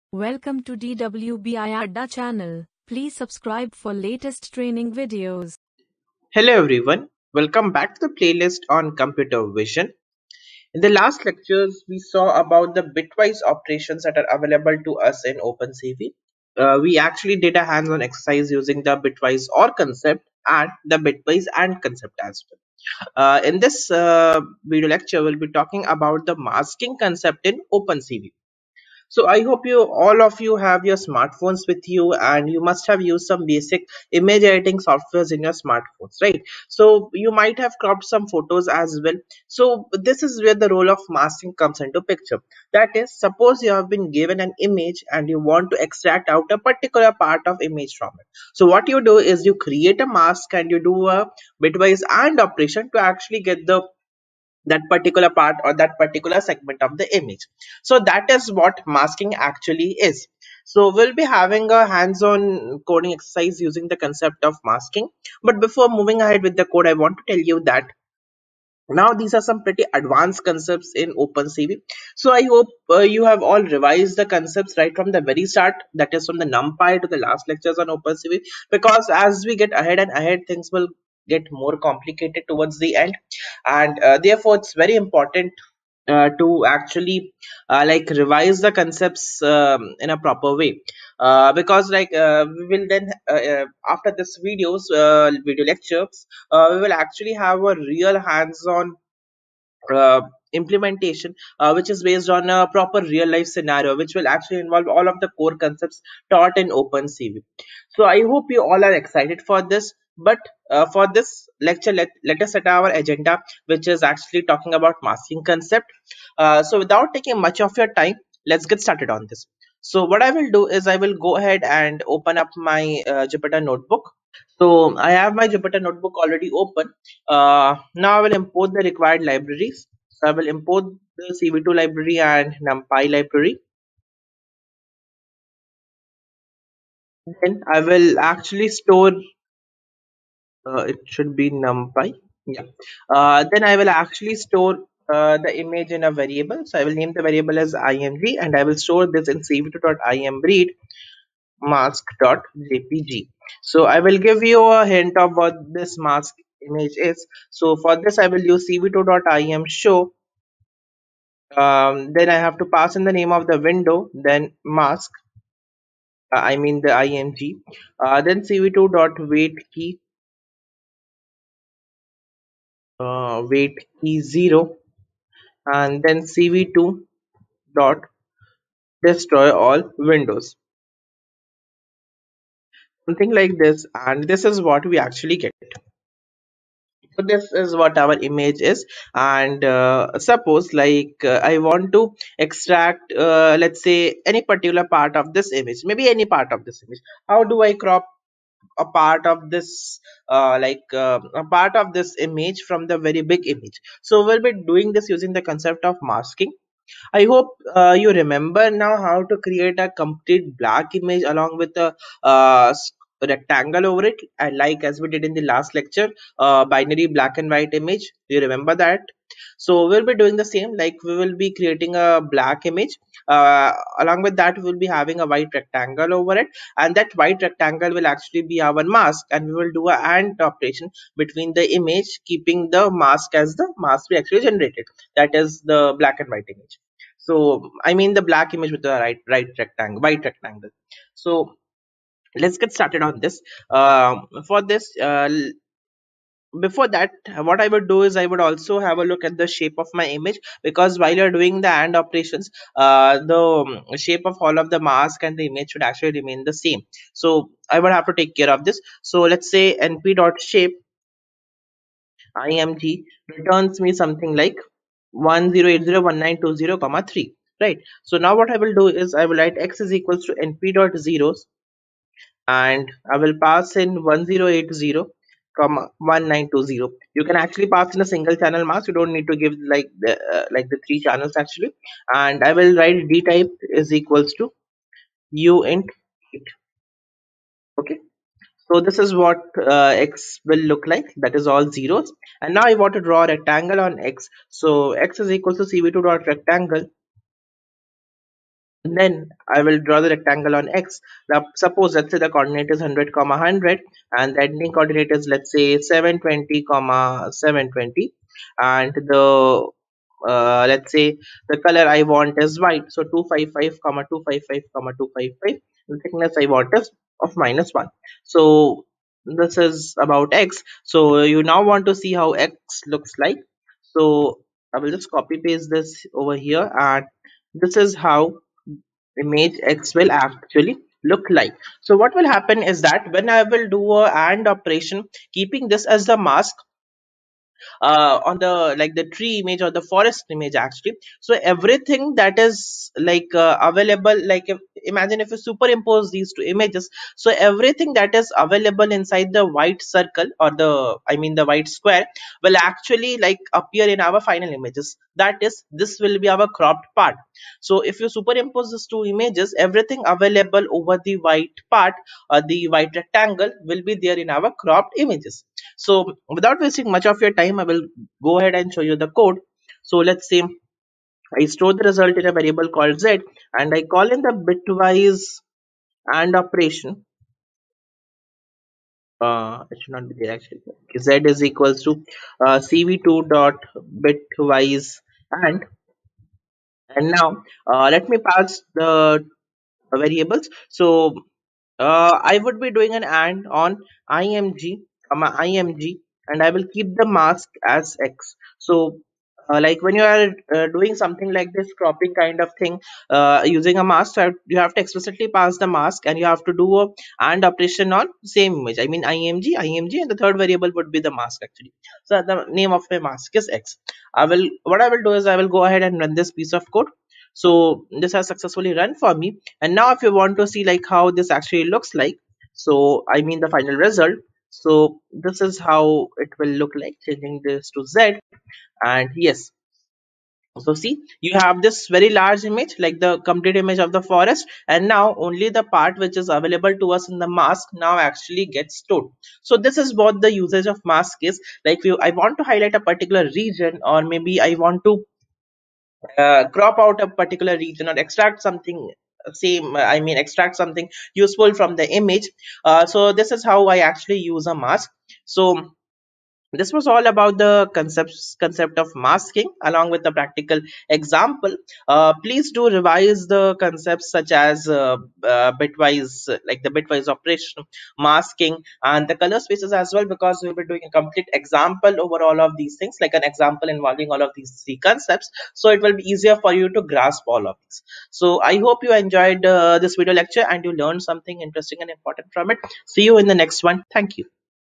Learn How to apply image masking in opencv in 8 mins | Opencv Tutorial
Welcome to DWBIADDA's computer vision ( OpenCV ) tutorial, as part of this lecture we are going  to   learn,Learn How to apply image masking in opencv